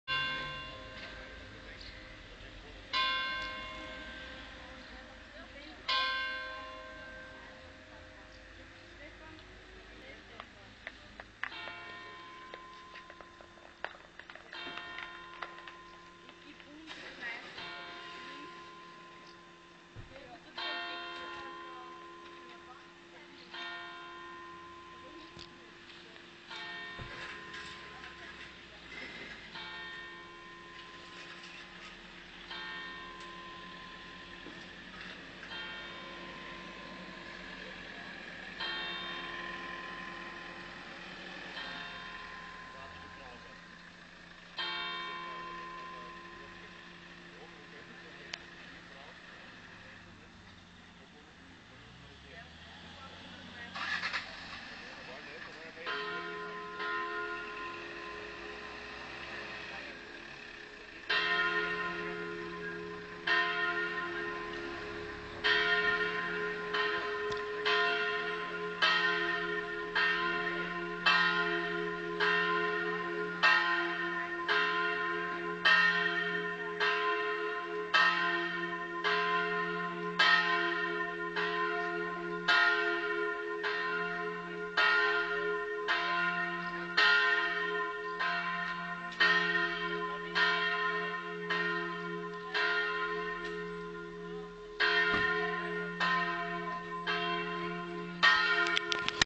Lockenhaus (A-7442) Pfarrkirche - Sonntagsmittagsangelus
Lockenhaus liegt im mittleren Burgenland im Bezirk Oberpullendorf.

f' a' c'' d'' - b''

GL 1,3,4 - St. Florian 1957
GL 2       - Herold      1669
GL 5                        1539

Normalerweise wird mit Glocke 2 der Angelus geläutet, außer an Sonntagen. Die Glocke hört sich auf dem Video leider nicht so gut an als wie am Hauptplatz.
Die Wartung übernimmt Schauer & Sachs.
Ich musste so früh abbrechen, weil ich abgeholt wurde.